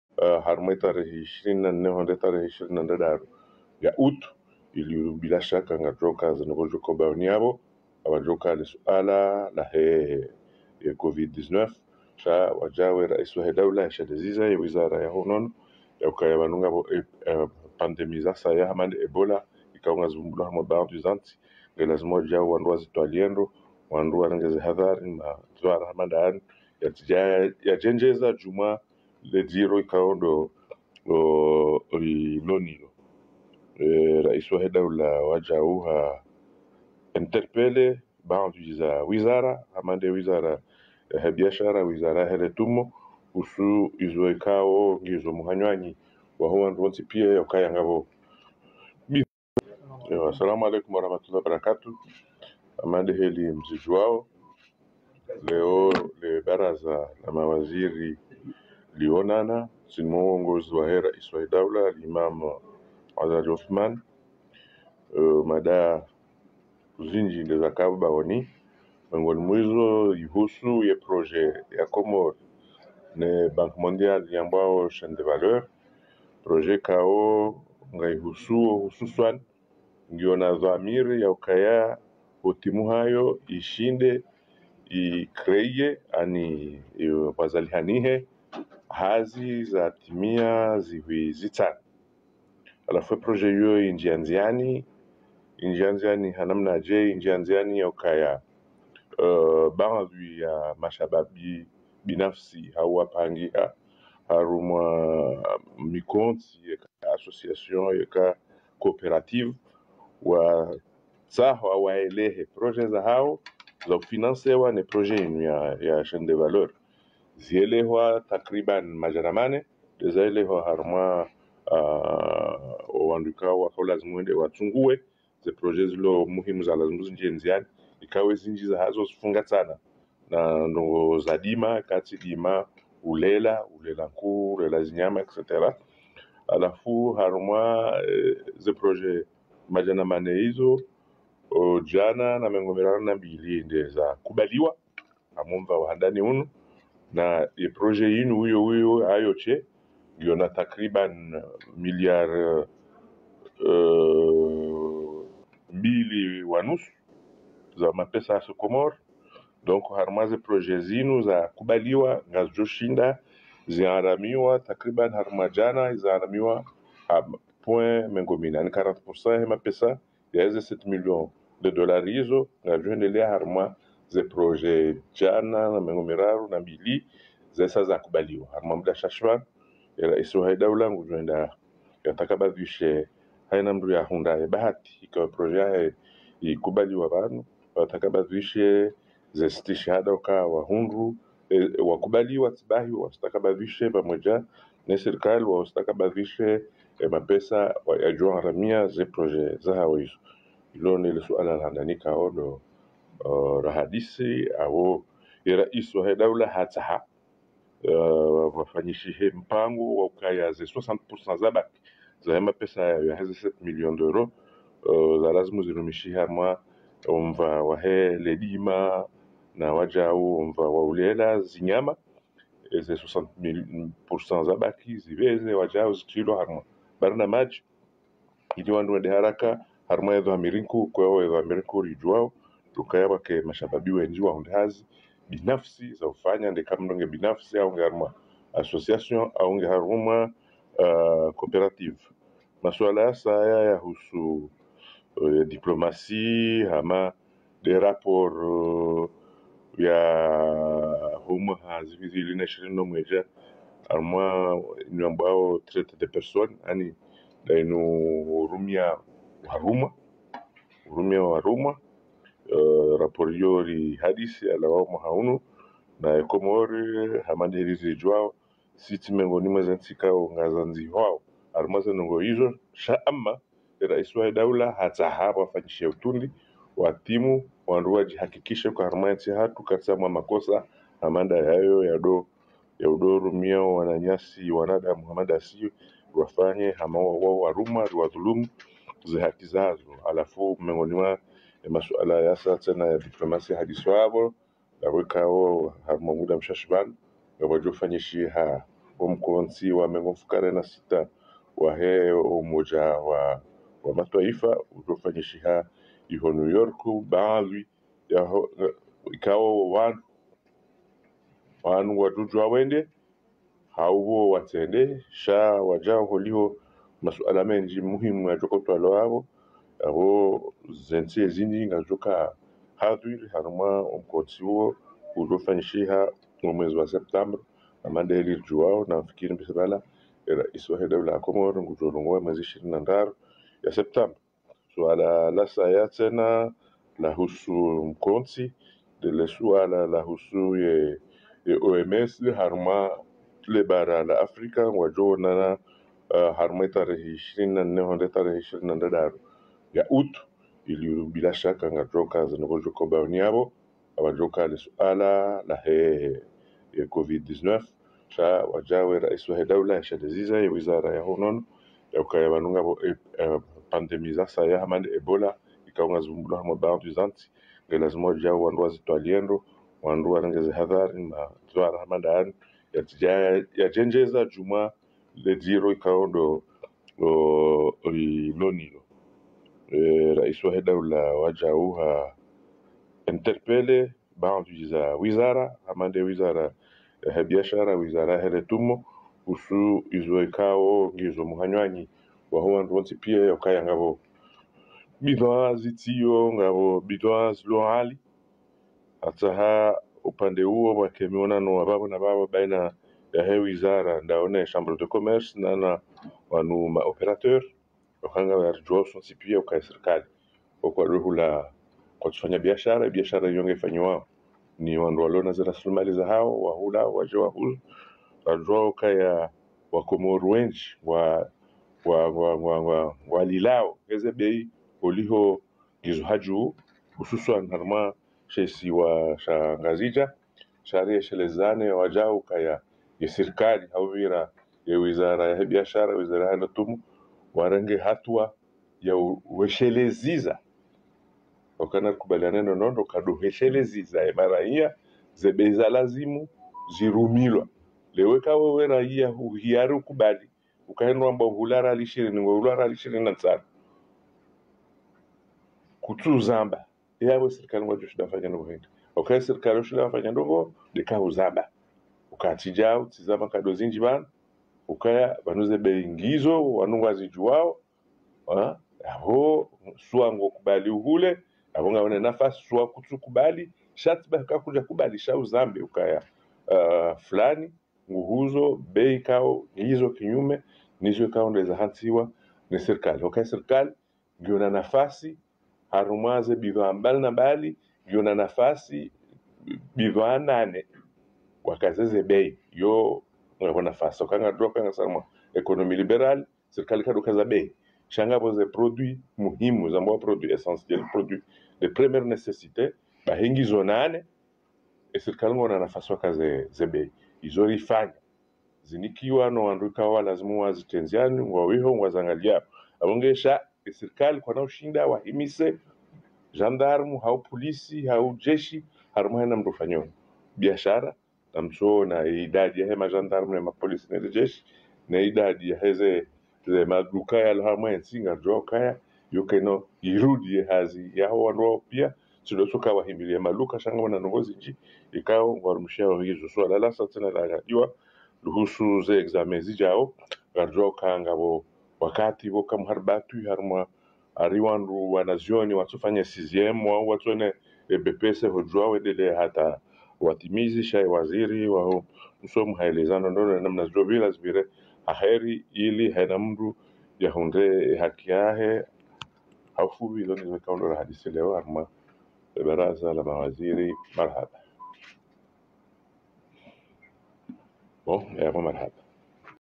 Point de presse du Ministre Msaidié
Point de presse du porte parole du gouvernement après le Conseil des Ministres - Al-watwan